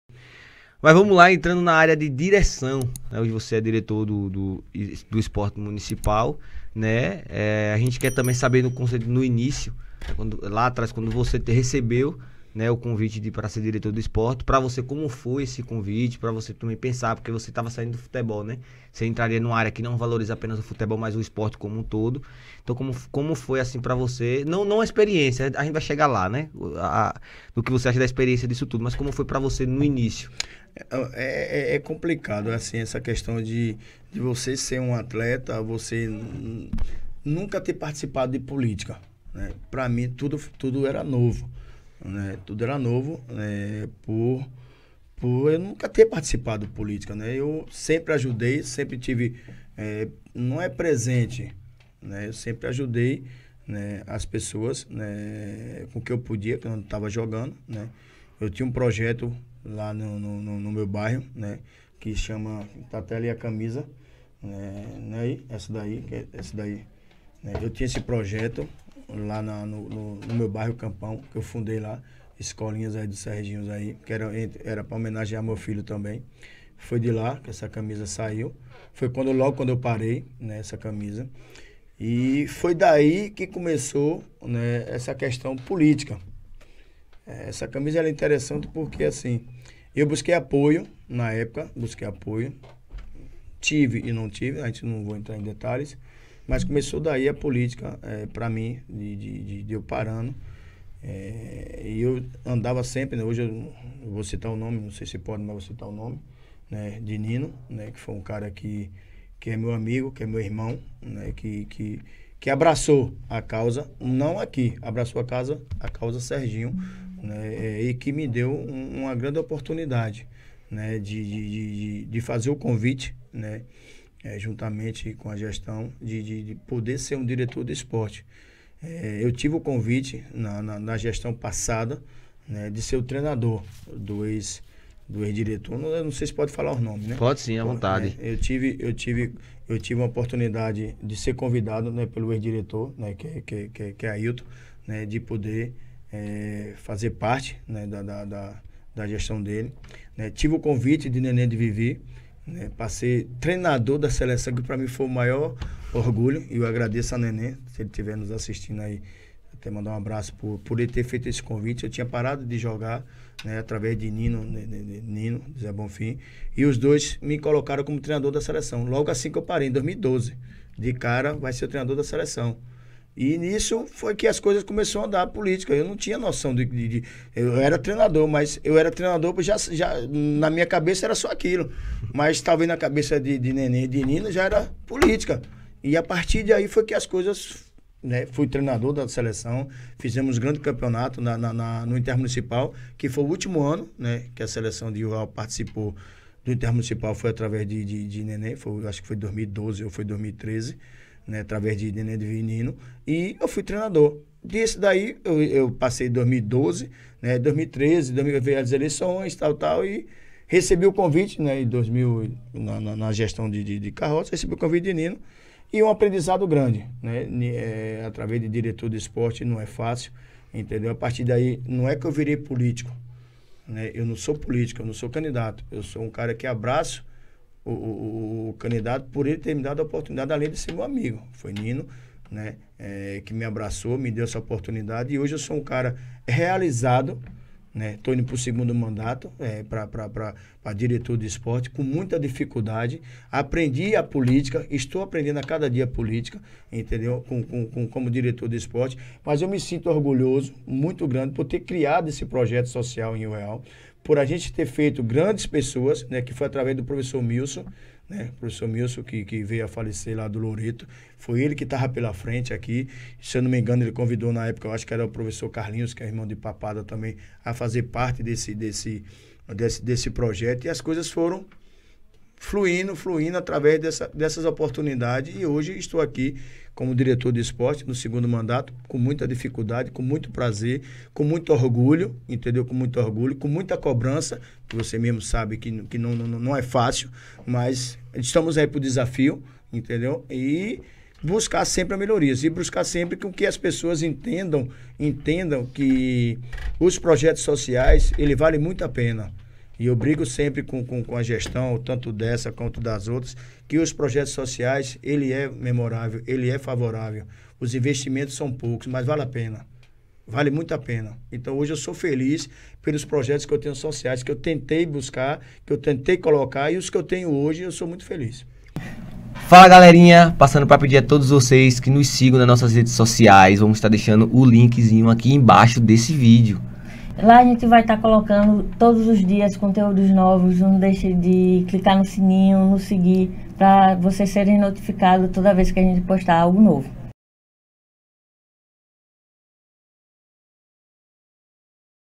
O convite para ser DIRETOR de ESPORTE de RIO REAL - Cortes Seu Podcast
Convidado: Serginho Baiano - Ex-Jogador de Futebol

Âncora: Celton Luiz